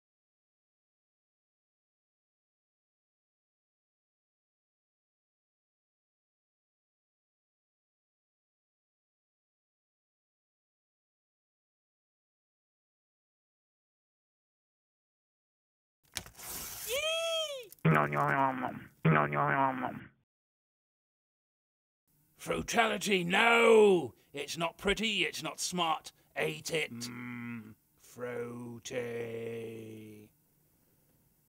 Kiwi .mov
A second year ADVP project - the fruit you can use as a pet - we went a little awry - but still . . . FRUITALITY! EAT IT!